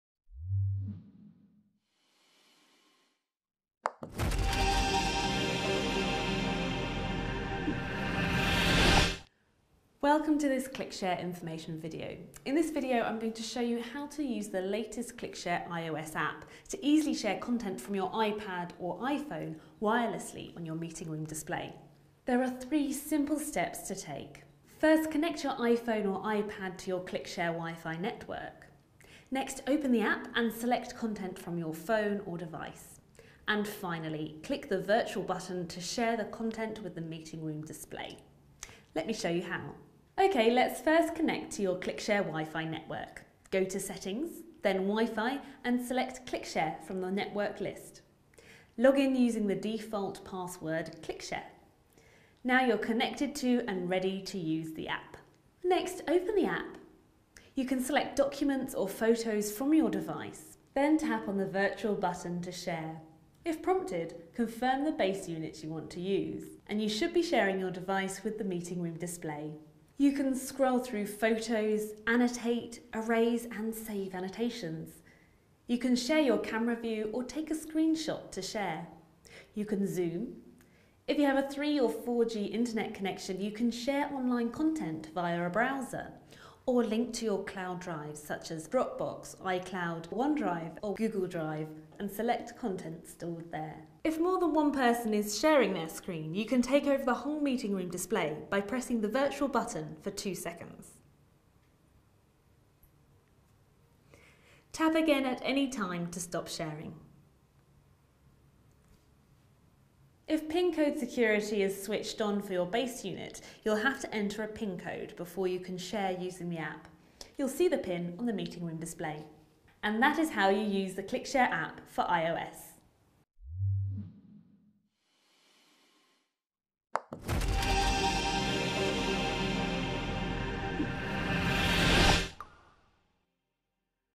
How to use the ClickShare iOS App
Connecting your iPad or iPhone to the central meeting room screen is simple and hassle-free with the ClickShare iOS App. This video takes you through the needed steps to get you started.